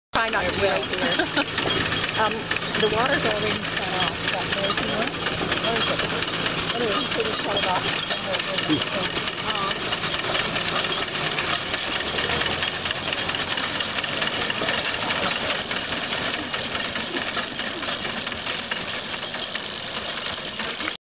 McCormick Binder demo at the Northville Farm Heritage Centre
From the Harvest Day machinery demo, 2011 (I think)